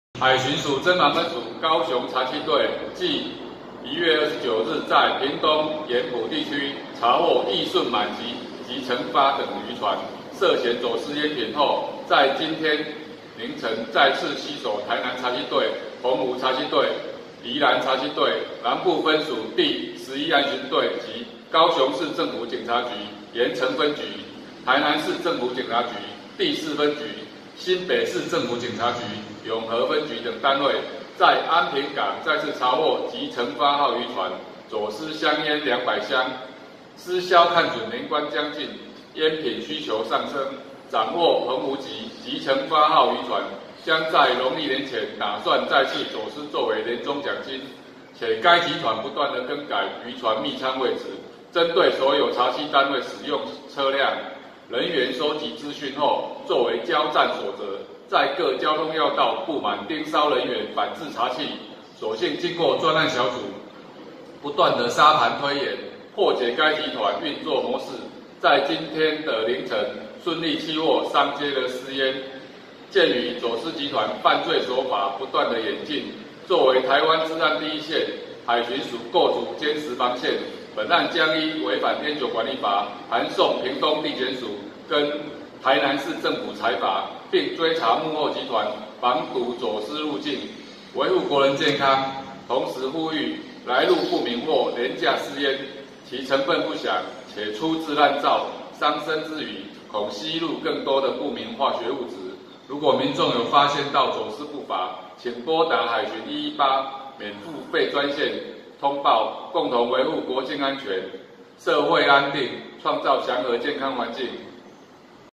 【私梟挺險再走私 海巡嚴查再嚴罰】環球日報社 World News Media
記者馮世光/高雄市報導

海巡署偵防分署高雄查緝隊，繼本(110)年1月29日於屏東鹽埔地區查獲溢○滿及吉○發等漁船涉嫌走私菸品後，於今(5)日凌晨再次攜手臺南查緝隊、澎湖查緝隊、宜蘭查緝隊、南部分署第11岸巡隊及高雄市政府警察局鹽埕分局、臺南市政府警察局第四分局、新北市政府警察局永和分局等單位，於安平港再次查獲「吉○發」號漁船走私香菸200箱(10萬包)。

私梟看準年關將近菸品需求上升，掌握澎湖籍「吉○發」號漁船將於農曆年前打算再次走私做為「年終獎金」，且該集團不斷更改漁船密艙位置，針對所有查緝單位使用車輛、人員蒐集資訊後做為教戰守則，在各交通要道佈滿盯梢人員反制查緝；所幸經過專案小組不斷沙盤推演破解該集團運作模式，於今日凌晨順利緝獲上揭私菸。

鑑於走私集團犯罪手法不斷演進，做為臺灣治安第一線，海巡署構築堅實防線，本案將依違反菸酒管理法函送屏東地方檢察署與臺南市政府裁罰，並追查幕後集團防堵走私入境，維護國人健康。同時呼籲，來路不明或廉價私菸其成分不詳且粗製濫造，傷身之餘恐吸入更多不明化學物質，如民眾發現走私不法，請撥打海巡「118」免付費報案專線通報，共同維護國境安全、社會安定，創造祥和健康環境。